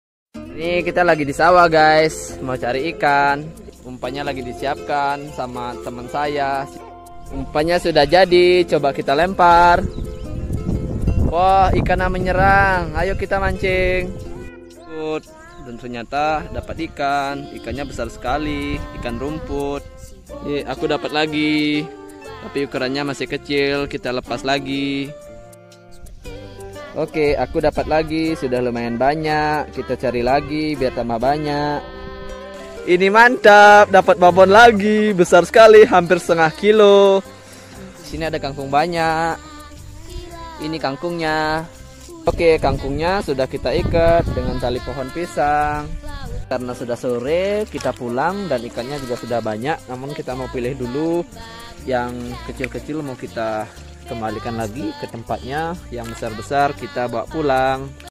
Panen Ikan di Sawah Banyak Ikan Mujair dan Ikan Nila
Panen ikan di sawah banyak ikan mujair

Hay temen" hari ini aku mau cari ikan, ikannya ada di sawah kebetulan aku pelihara ikannya disawah yg sudah aku buatkan kolam.